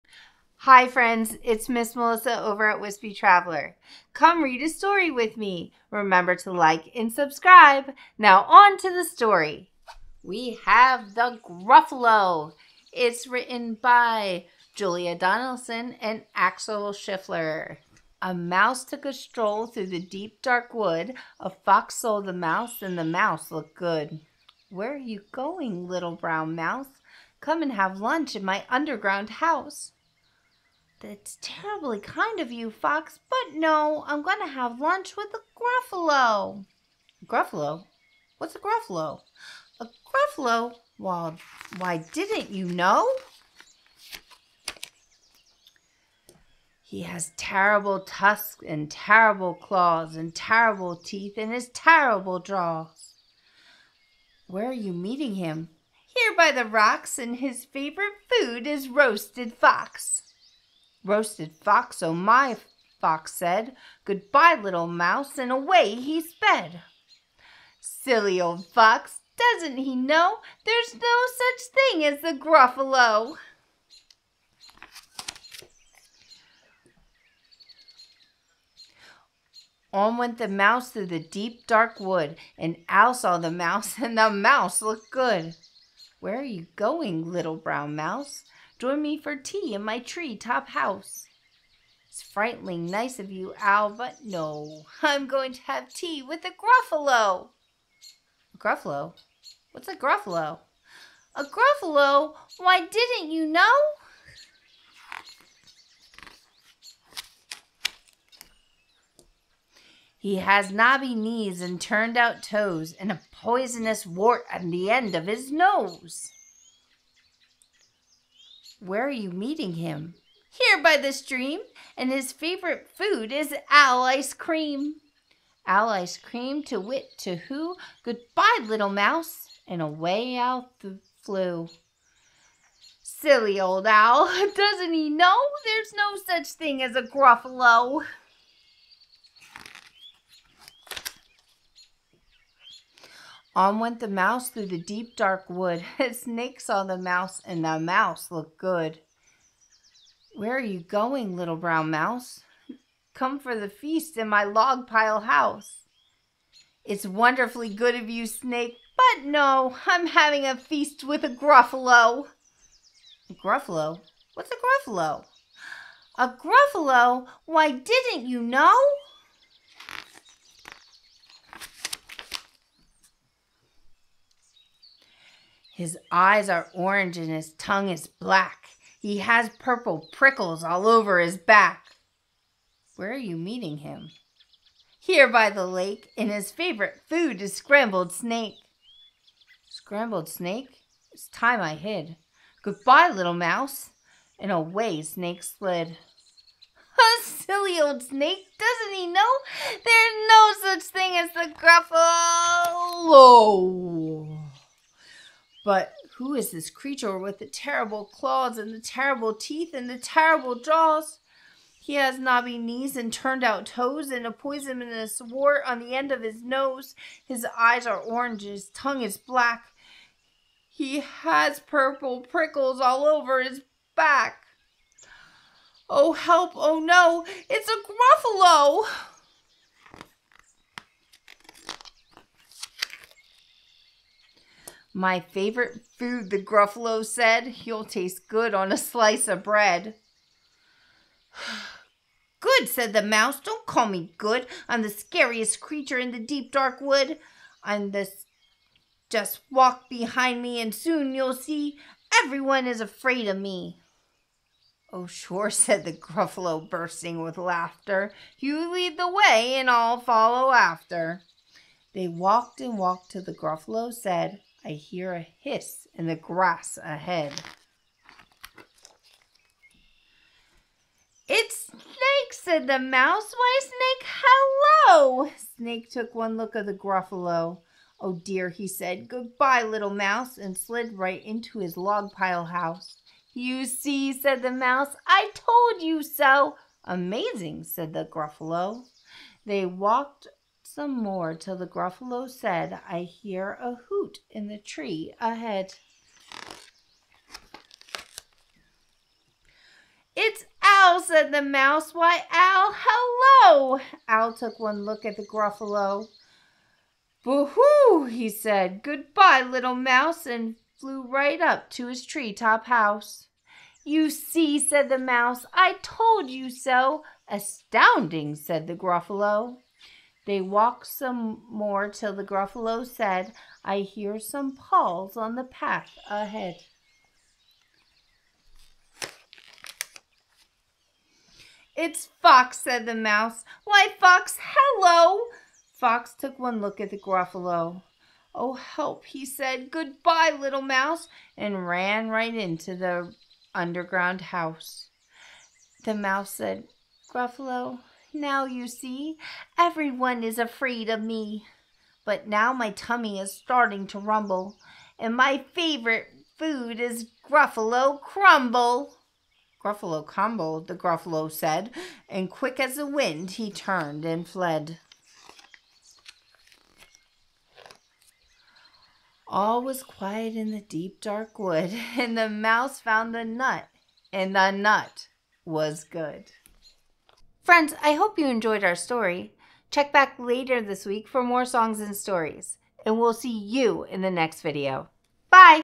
Gruffalo - Read Aloud withme-Episode 14
Hi Friends! Its time to read a story! Come on in and join me. Today we are going to read 'The Gruffalo' written by Julia Donaldson. Illustrations by Axel Scheffler.